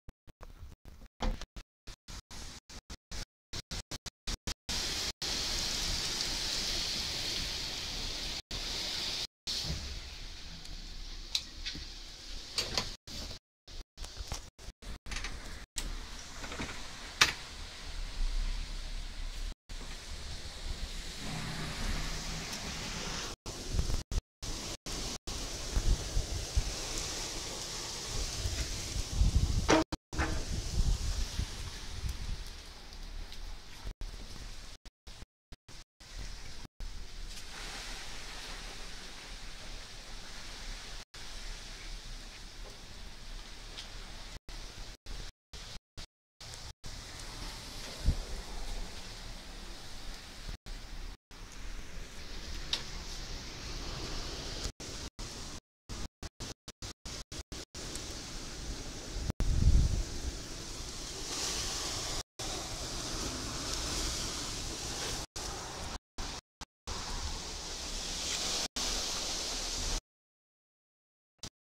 The Strom with cloudy sky at al wadi wej street in KSA
This video was taken by Tamil guy.(madharasi).